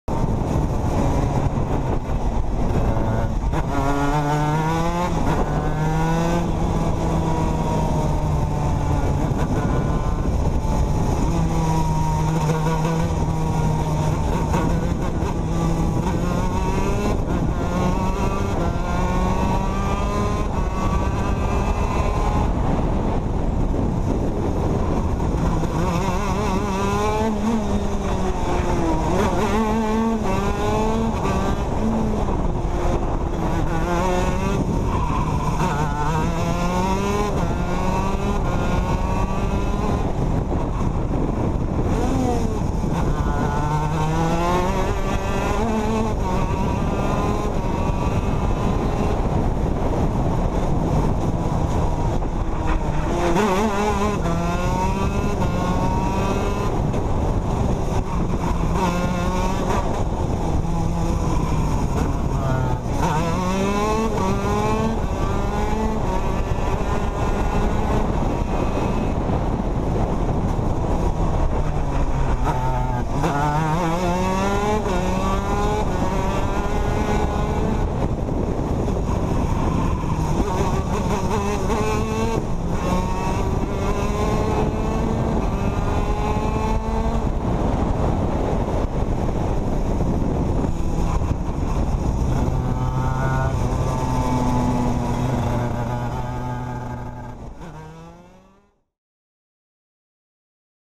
F1 Outdoor Hot Lap
After a season of failed engines I retired the CR125 for the day and took out my buddy's TM Racing kart. This was my fastest lap of the day @ 1:06. Still figuring out the kart and the track, plenty of room for improvement.